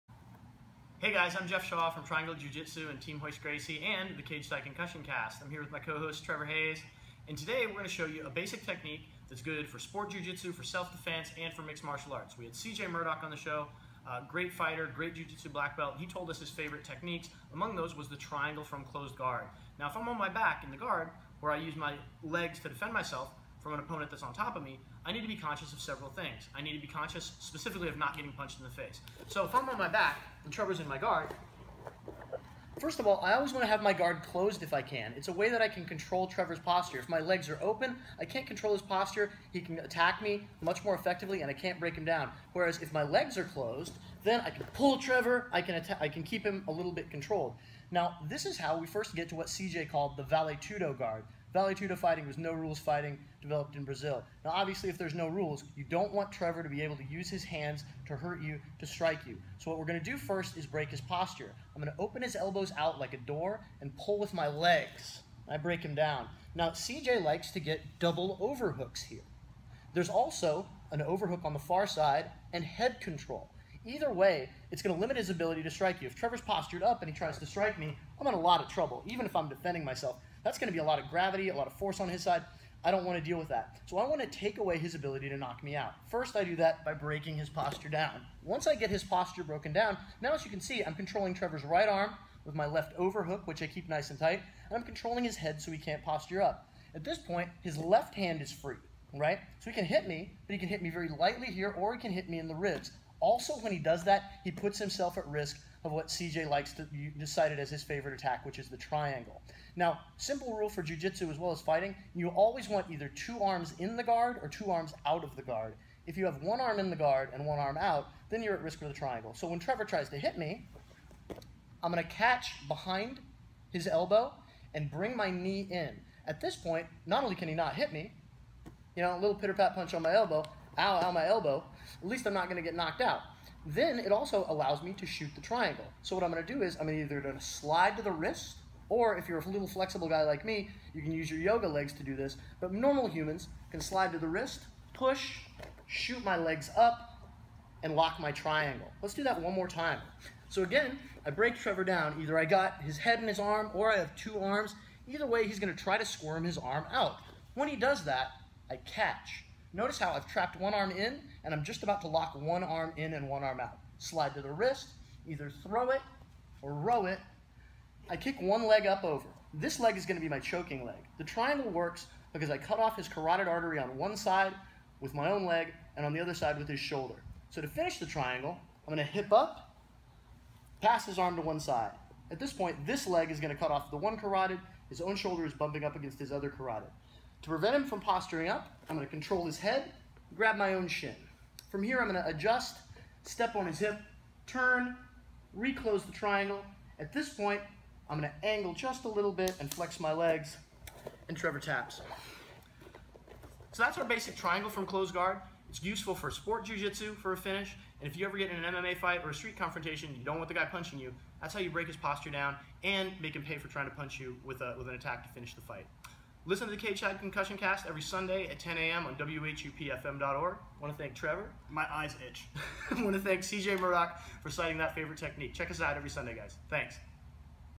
Triangle From Closed Vale Tudo Guard Instructional
On the Oct. 25 Cageside ConcussionCast, CJ Murdock cited this technique as one of his favorites. Jeff Shaw, a purple belt from Triangle Jiu-Jitsu Durham & Team Royce Gracie shows how to do it. Listen to the show Sundays, 10 a.m. on WHUPfm.org, or download the podcast afterward at WHUPfm.org.